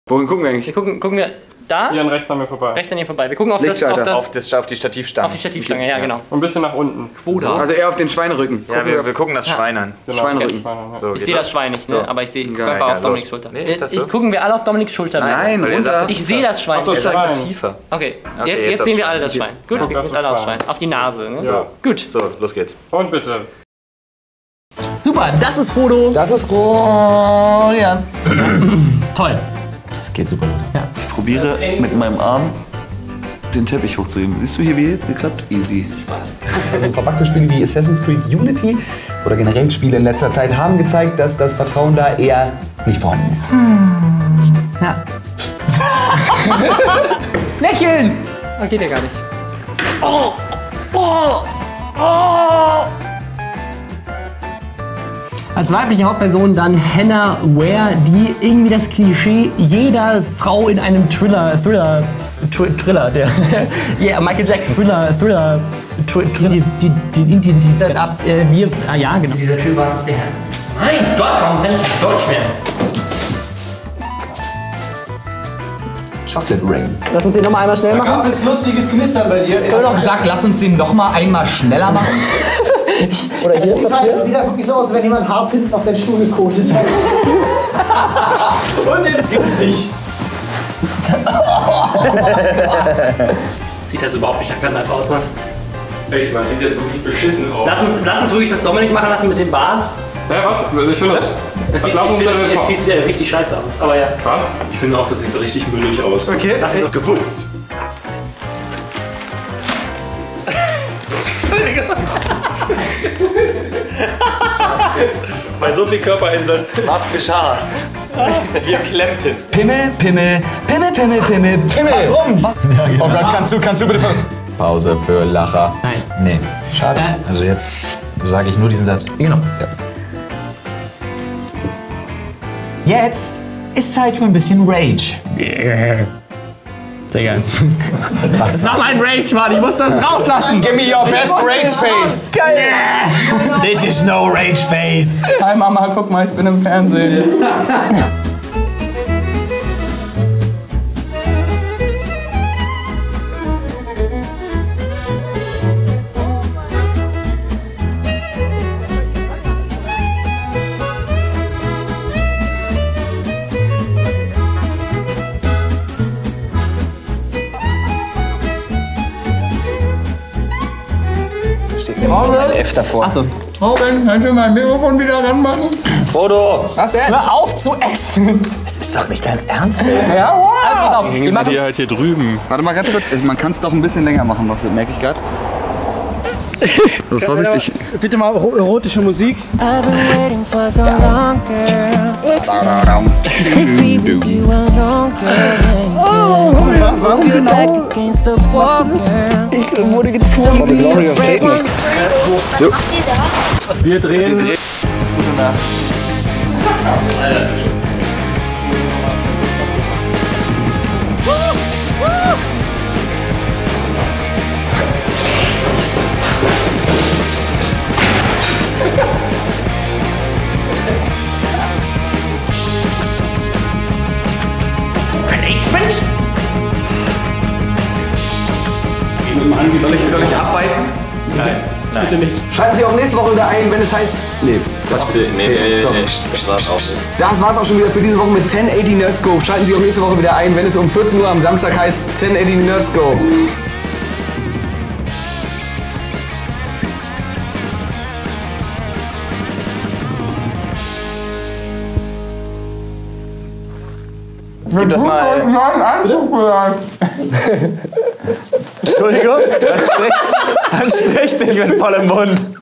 Totgelacht & kaputtgeekelt - 1080Outtakes No. 1
Wer sind DOKTORFROID eigentlich?

Haben euch heute ja gefragt, ob ihr gerne ein Outtake-Video hättet… und ihr hättet gern. Also: Bitteschön. xD
Ab Donnerstag dann wieder Gaming-Videos! Cheerio!! :D